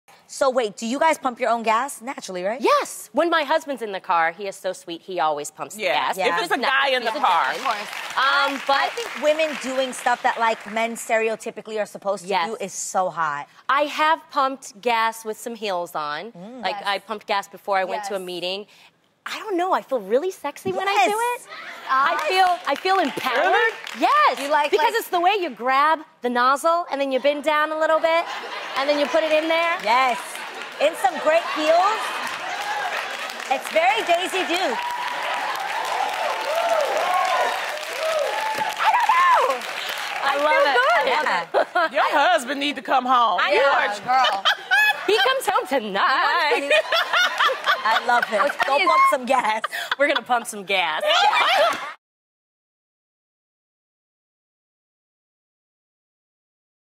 Why Tamera Is Excited to Pump Gas
Tamera shares the hilarious reason she's excited to "pump gas" with Adam!